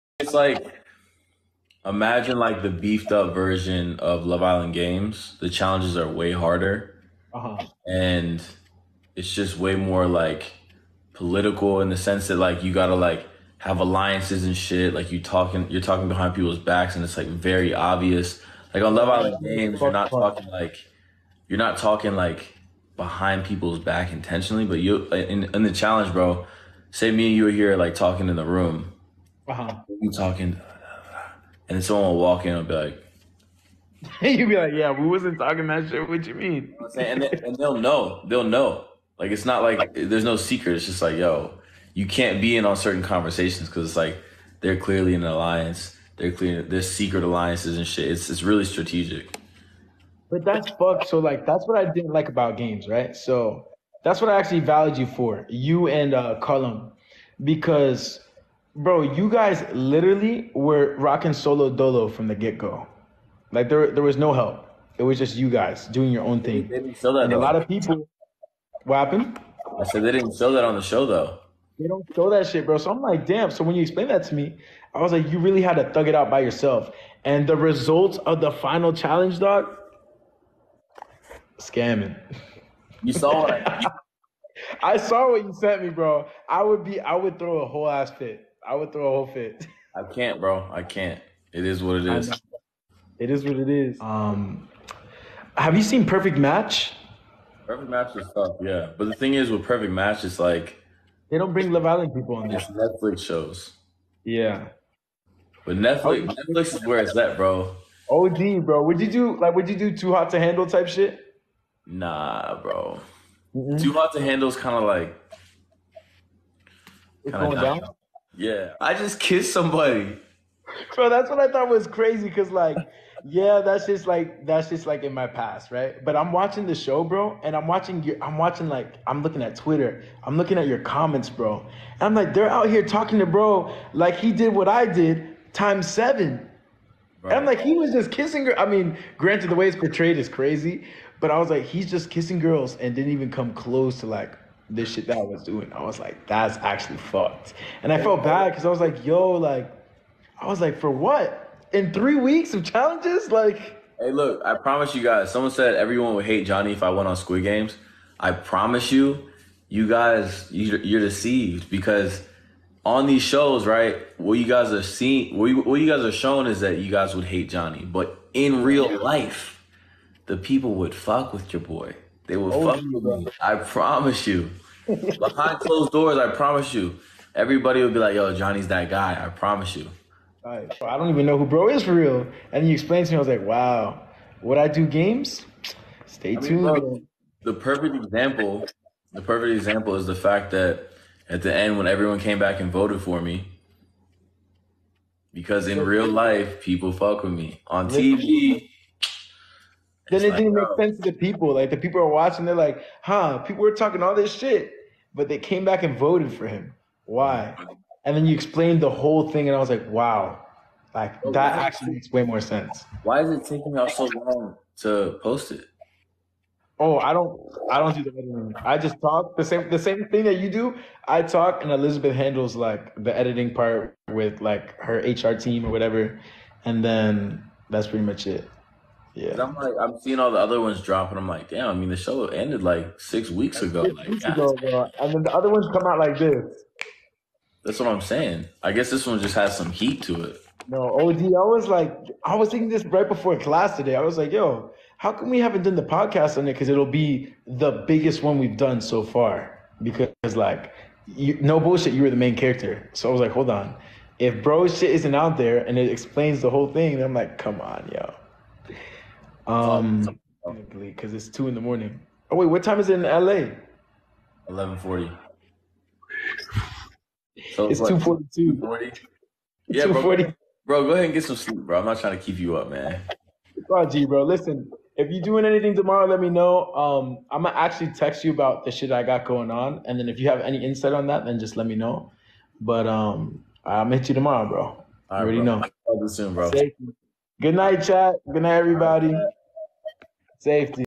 Love Island Games S1 | JOHNNY MIDDLEBROOKS says who the REAL winner is | VILLAIN edits with LEO
Johnny Middlebooks LI USA S2 +talking LIG S1 talks with Leonardo Dionicio LI USA S5  about Love Island Games.

In this video they talk about about the show The Challenge and difference between it and Love Island Games.

Johnny reiterates that he actually won not Justine and Jack.

They discuss reality shows they would/ would not go on Perfect Match, Too Hot to Handle.

Johnny and Leo compare villain edits

Johnny Middlebrooks: IG | johnny_llee, Tik Tok | johnny_llleee

Leonardo Dionicio: IG leonardo_dionicio, Tik Tok | leonardo_dionicio